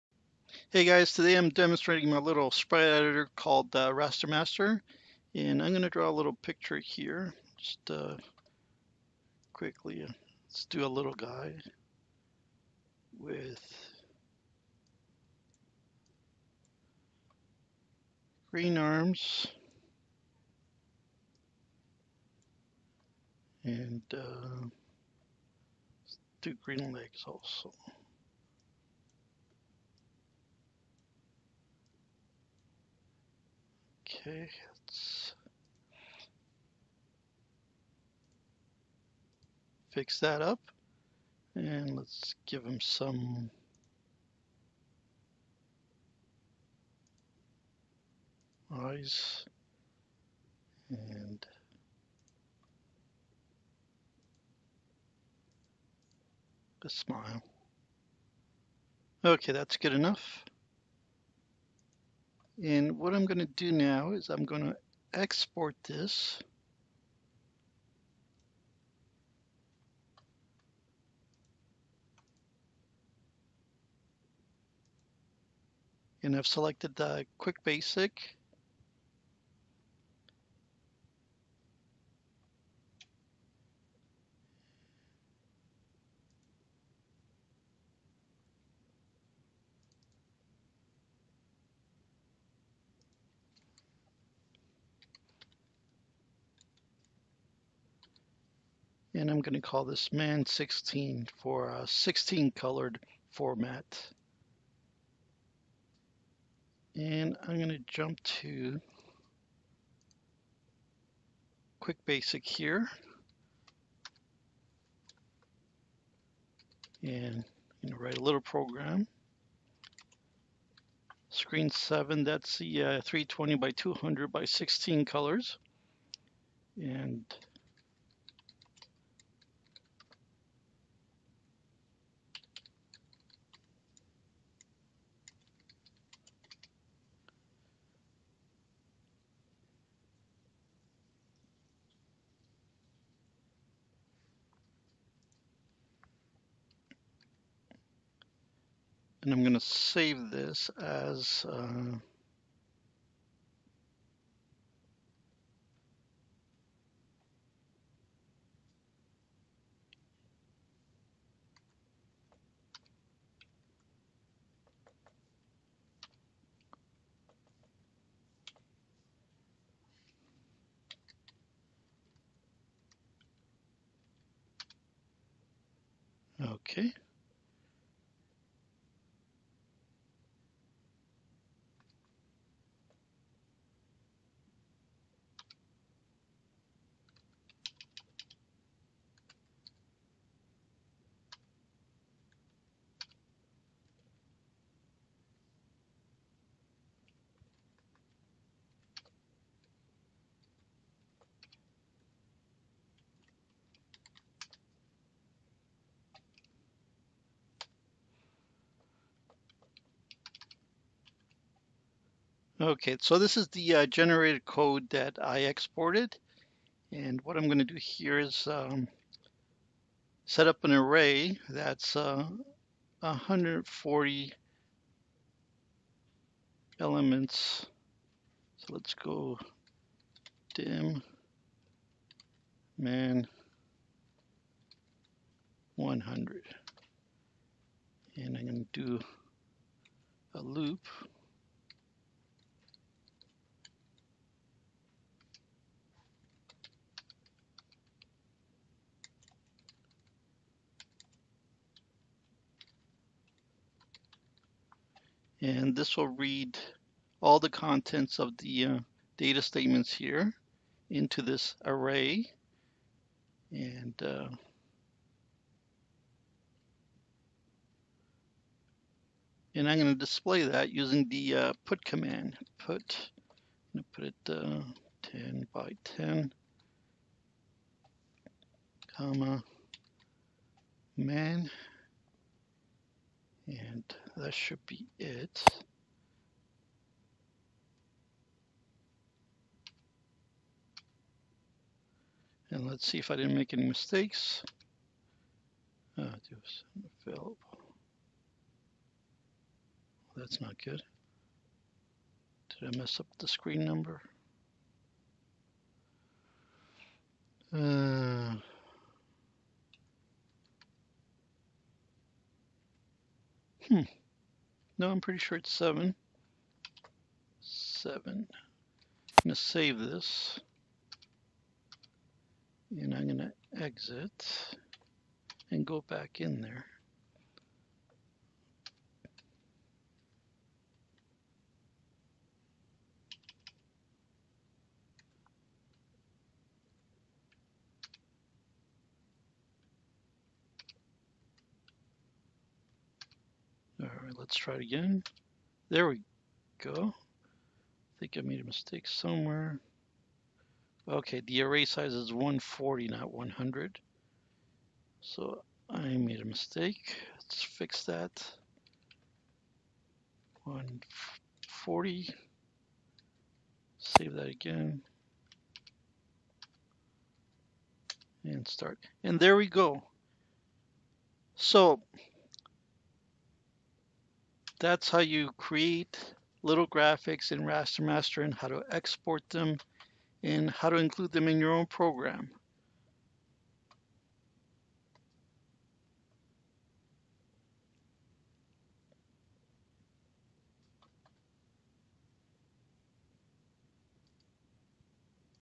✅ Raster Master Export Graphics to QBasic/QuickBasic/QB64 - Day 0
In this video i show you how to draw something in Raster Master sprite editor and export image to QuickBasic.